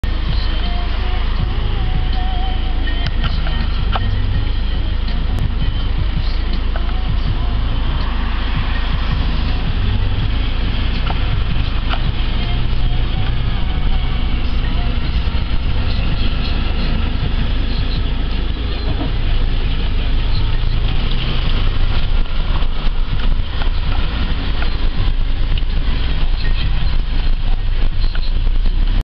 Iveco Magirus FF150 E 30W Feuerwehr Bautzen
Film jest mojego autorstwa został nagrany w miejscowości Bautzen ( Niemcy ) podczas postoju na stacjii paliw : )